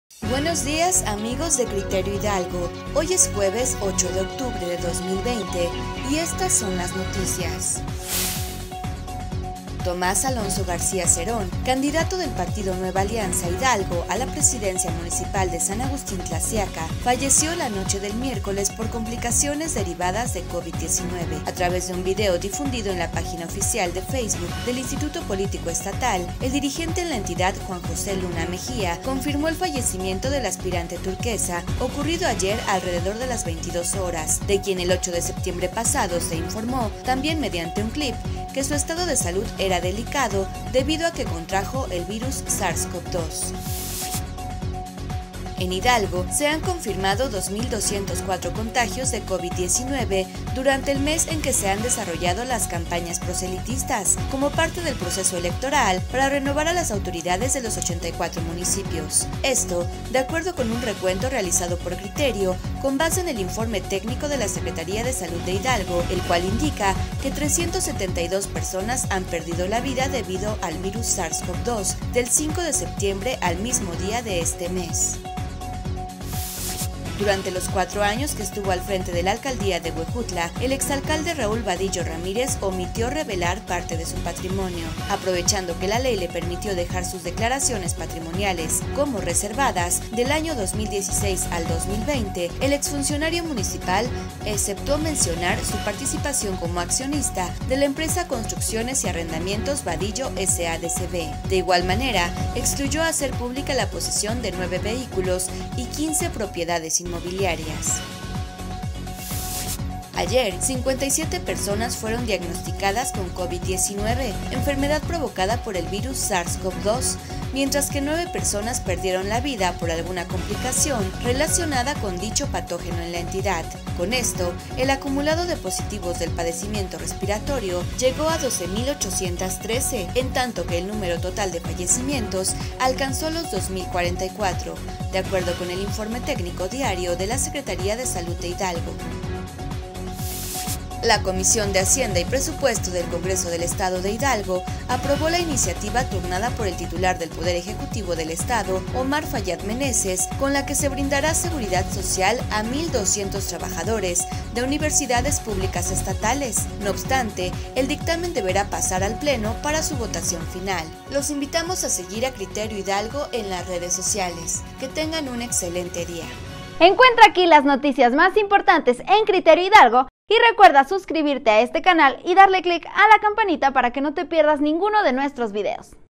Excelente día, no te pierdas las noticias más importantes en Criterio Hidalgo
▶️ Muere por Covid-19 el aspirante del Panalh a la alcaldía de San Agustín Tlaxiaca
▶️ En un día, 9 muertes y 57 nuevos positivos de Covid en Hidalgo
🔴Descarga la versión impresa de Criterio Hidalgo